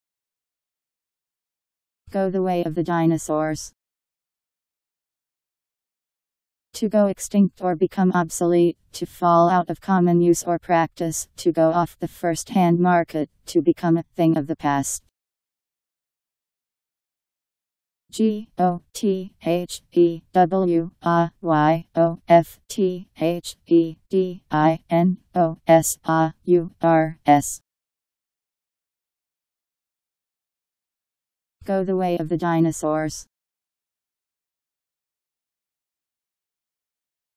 Go the way of the dinosaurs Meaning
Video shows what go the way of the dinosaurs means. to go extinct or become obsolete; to fall out of common use or practice; to go off the firsthand market; to become a thing of the past.  Go the way of the dinosaurs Meaning. How to pronounce, definition audio dictionary. How to say go the way of the dinosaurs. Powered by MaryTTS, Wiktionary